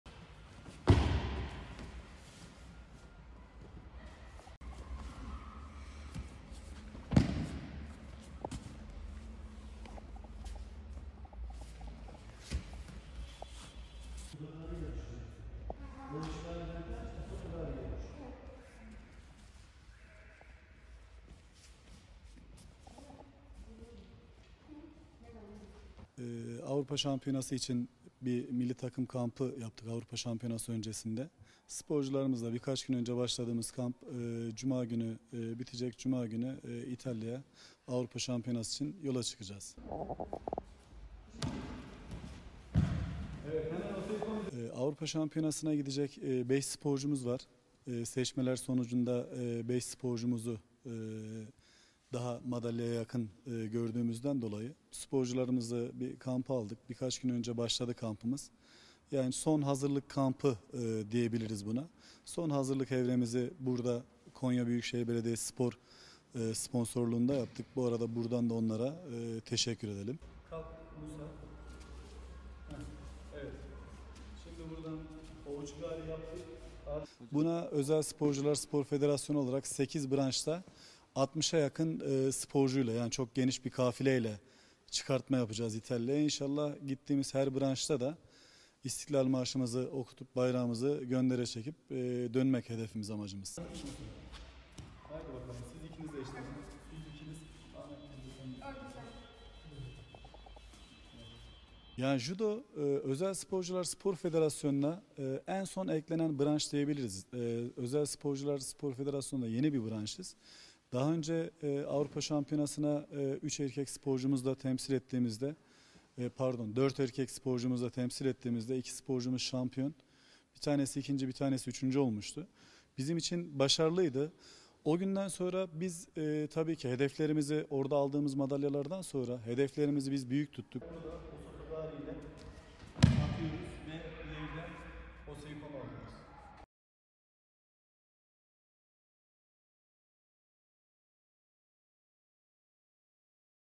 Özel Sporcular Down Judo Milli Takımı Konya'da kampa girdi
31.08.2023
Özel Sporcular Federasyonu Judo Milli Takımı Antrenörü Hamza Selen, AA muhabirine, Avrupa Şampiyonası öncesinde Konya Büyükşehir Belediyesi yardımlarıyla kampa girdiklerini belirtti.